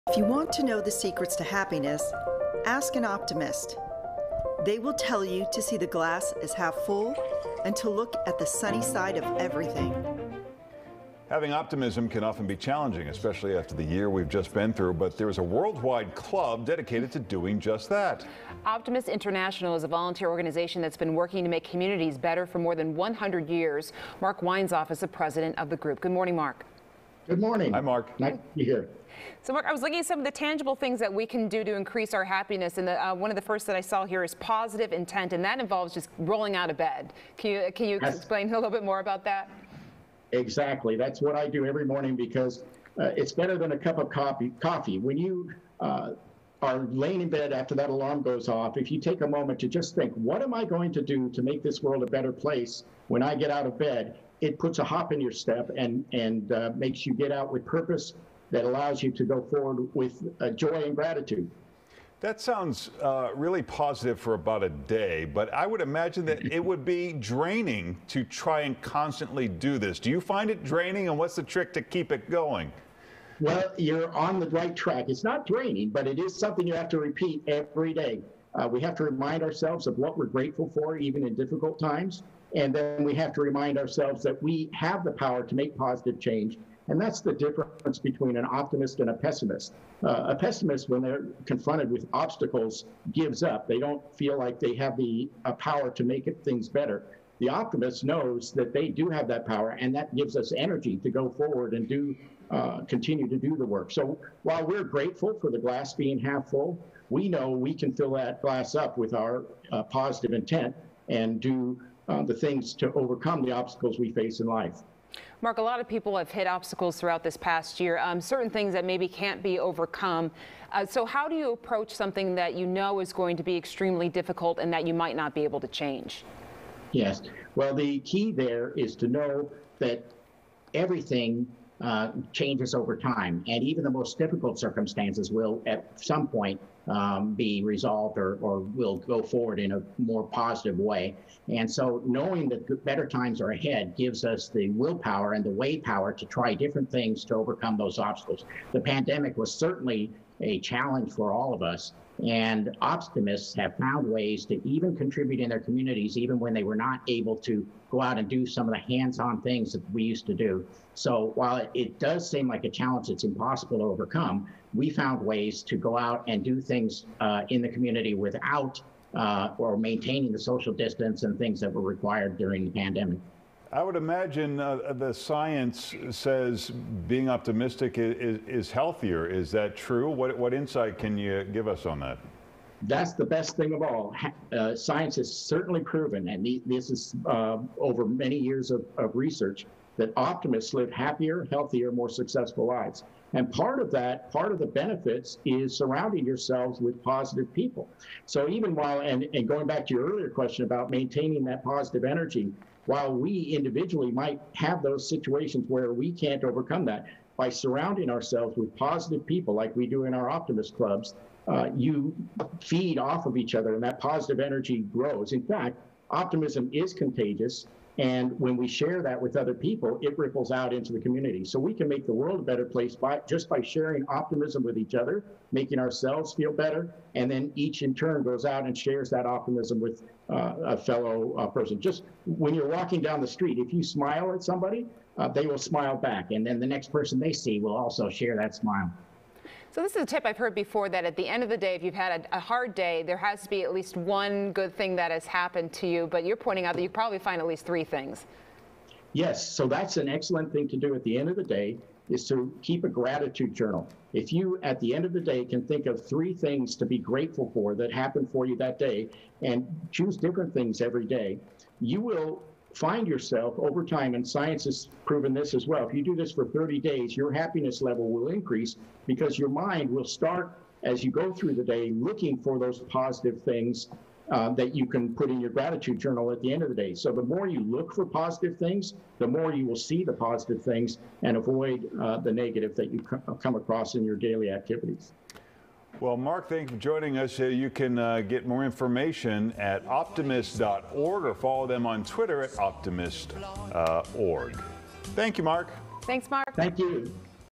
Optimist Clubs See Glass Half Full Even in a Pandemic
Mark Weinsoff, President of Optimist International